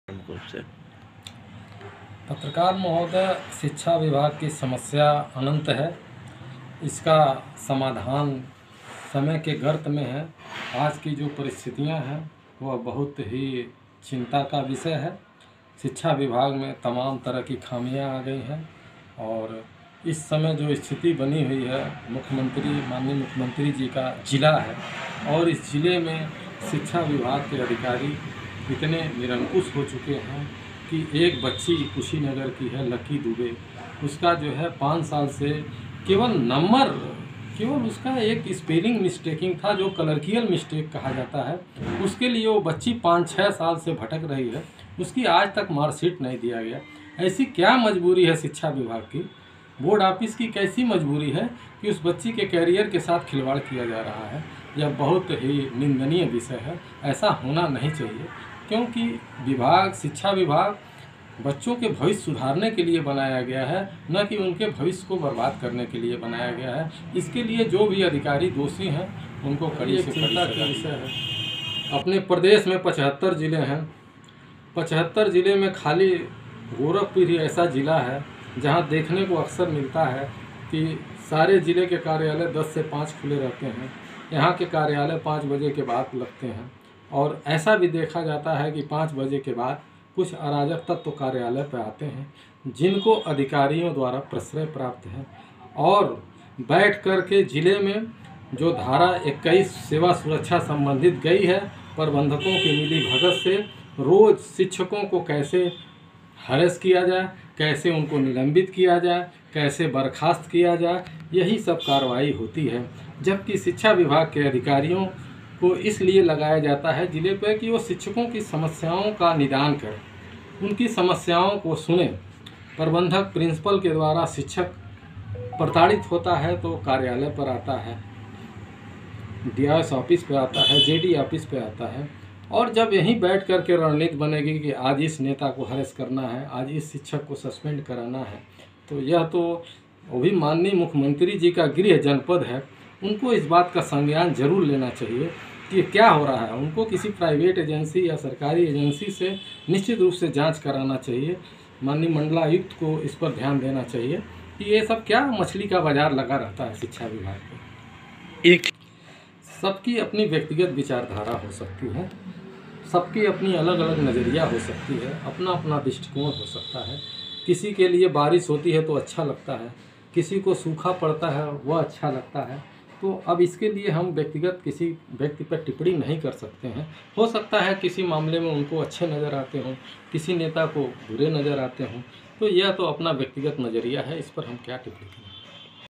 गोरखपुर शिक्षा विभाग ऑफिस का सिस्टम ठीक- नहीं |बच्चों के भविष्य के साथ खिलवाड़ किया जा रहा है
ब्रेकिंग गोरखपुर शिक्षा विभाग ऑफिस का सिस्टम ठीक- नहीं चल रहा है और बच्चों के भविष्य के साथ खिलवाड़ किया जा रहा है